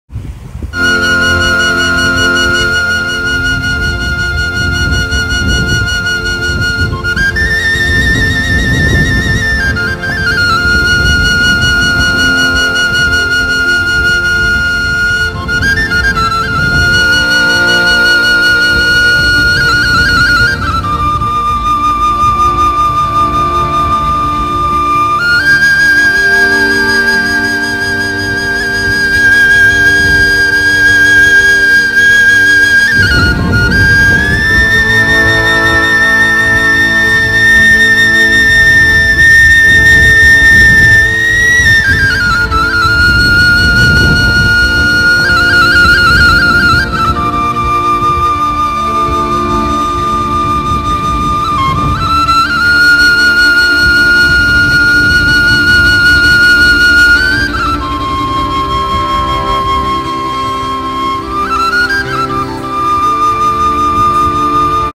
Water Circle Waves Device
Different recording angle.
Testing invention prototype device for making continuous concentric waves on pond, pool, lake, marinas ...

Music Bora Dugic, Serbian virtuoso on a fife, wooden flute, which make a tone continuous without pause, circular breathing technique.